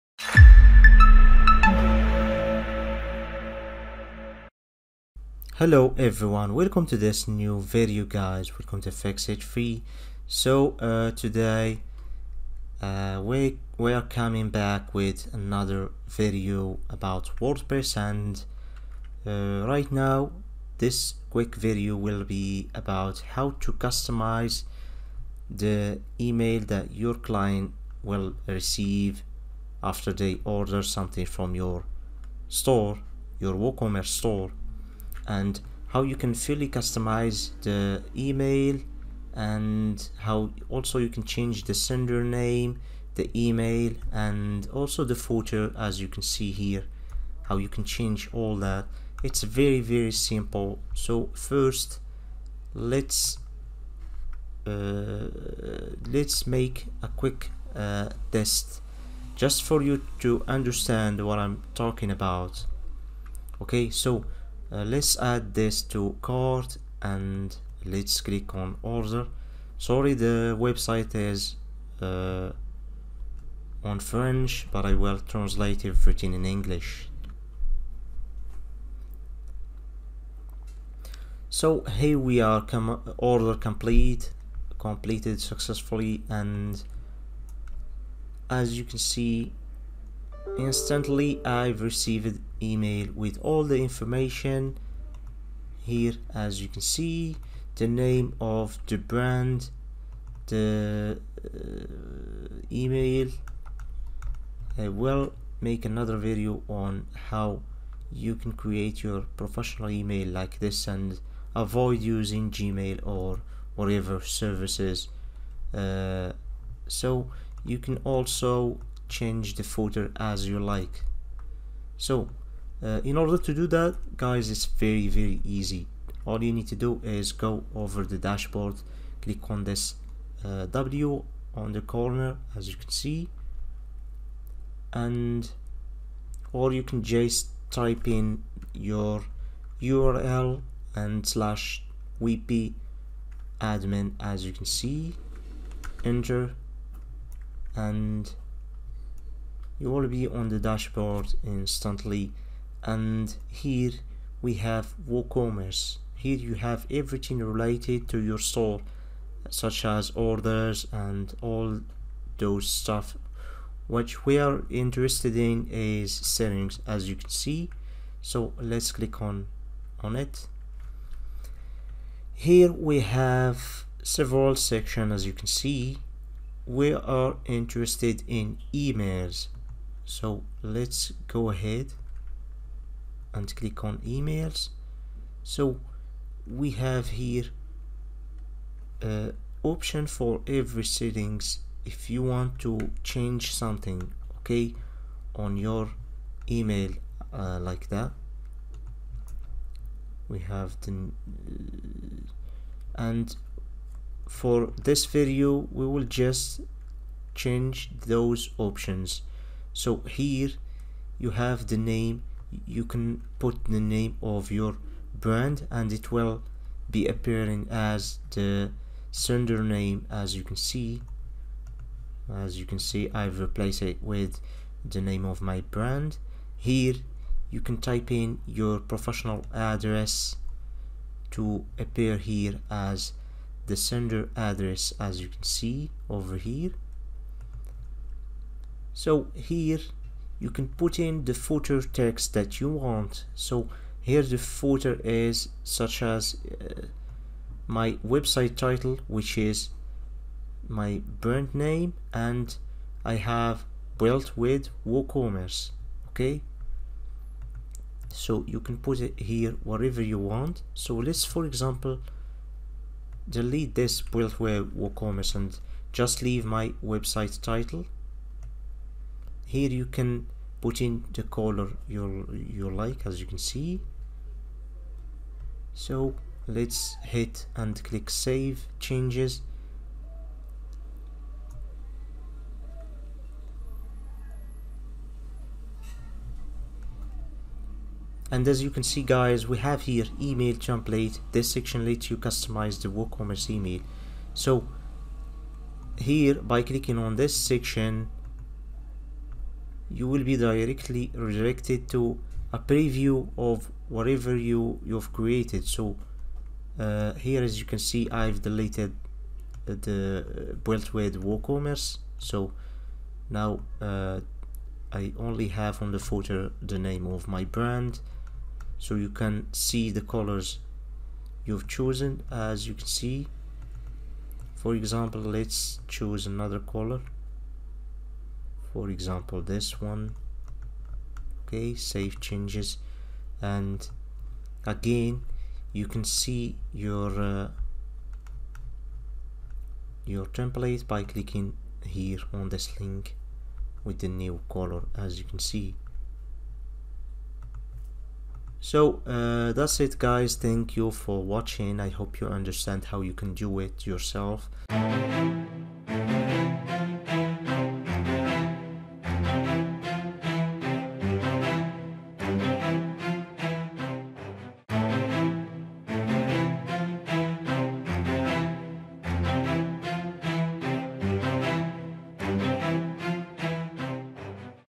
how to Change the Default From Name and Email in WooCommerce
Let’s collaborate and bring your ideas to life 🚀 By default, any emails sent from WordPress would have the default name as “WordPress” or "MY BLOG" and the from email would look like “ “
and if you don't change the settings every client will receive the order notification email with the default sender name and email.